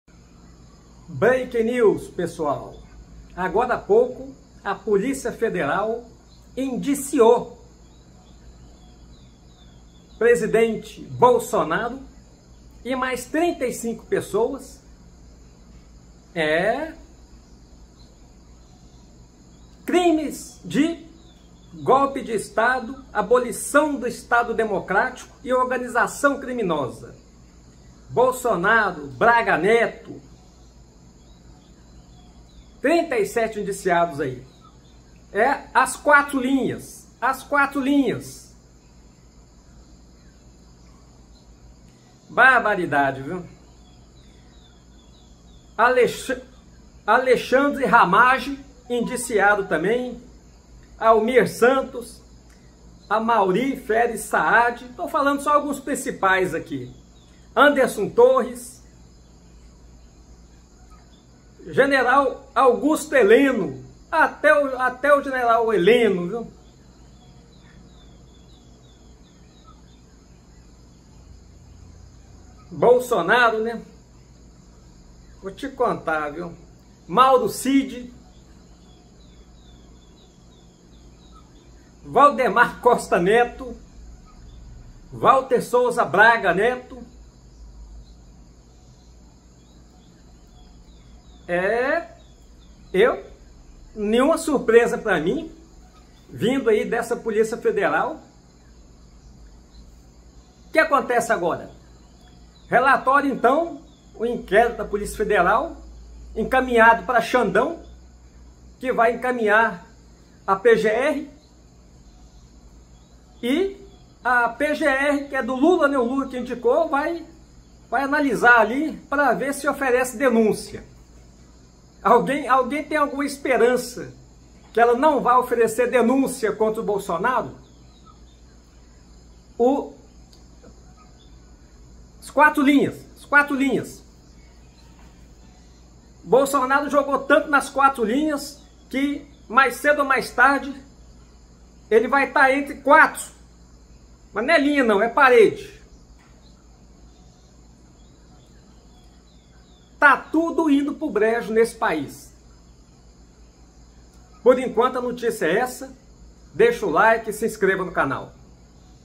URGENTE - Bolsonaro é indiciado pela Polícia Federal
Bolsonaro é indicado pela polícia federal.